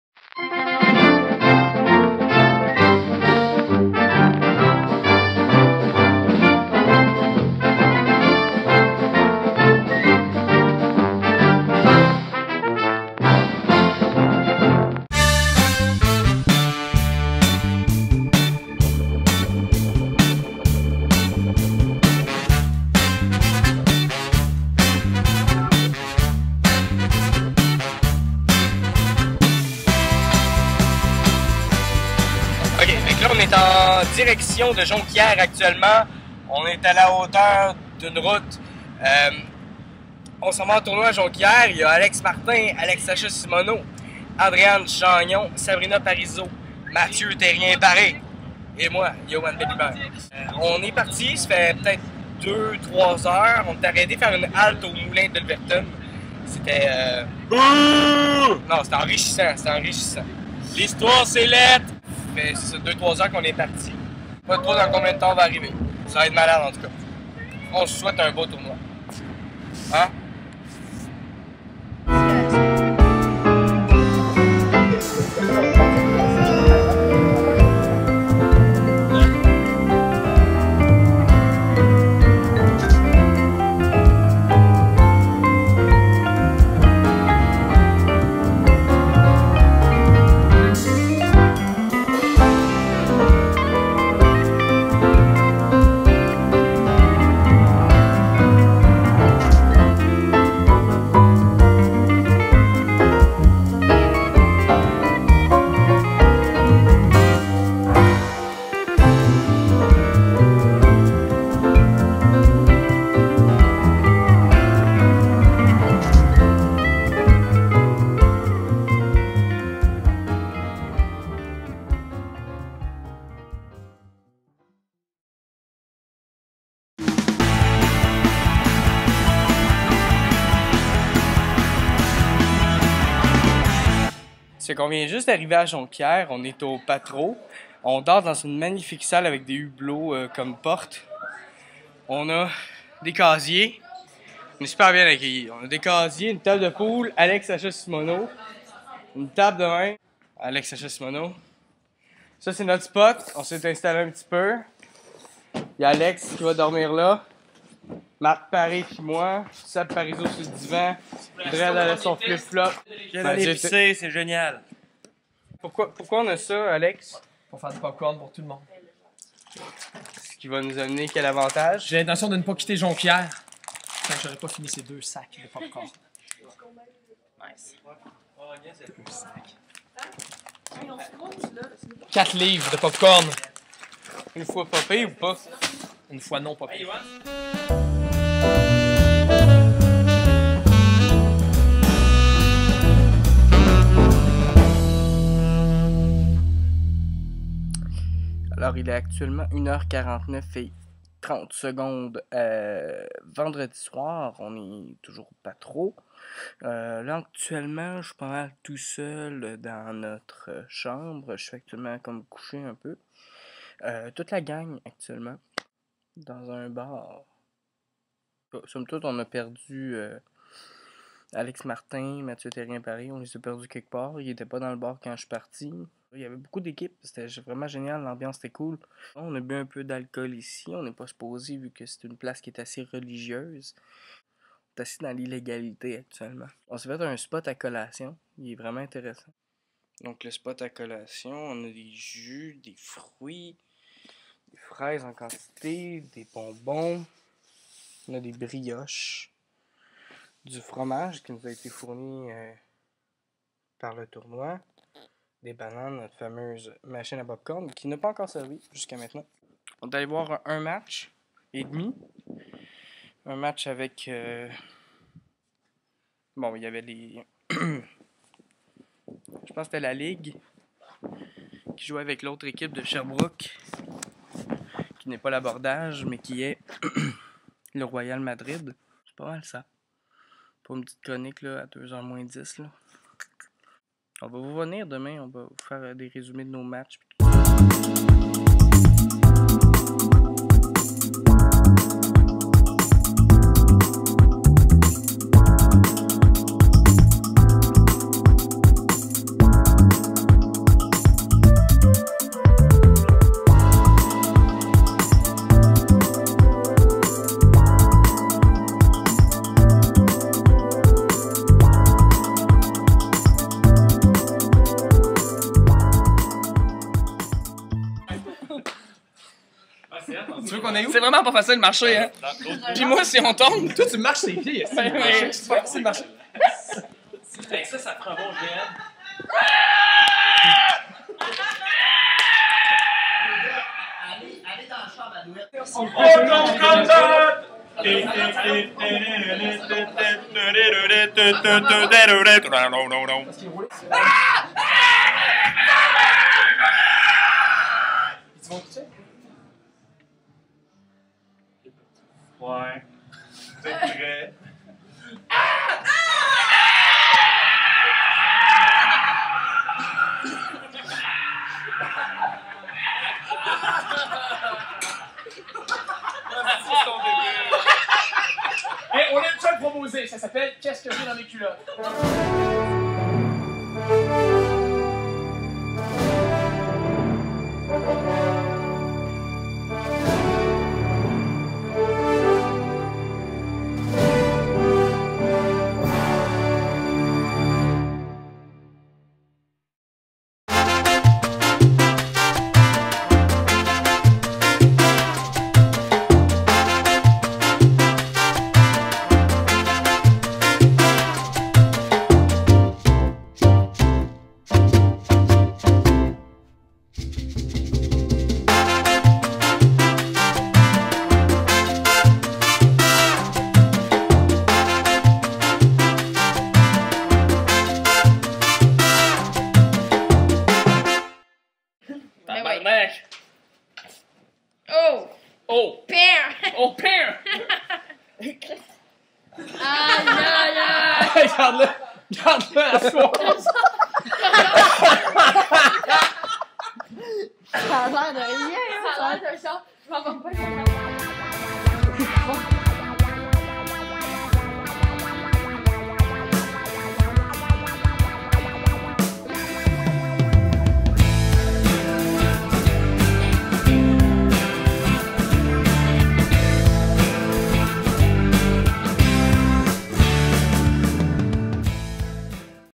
L'Abordage au Patro de Jonquière
L'abordage fait son envolée vers le Patro de Jonquière et s'embarque dans un 2 jours de purs plaisir entre improvisateurs venu de partout au Québec.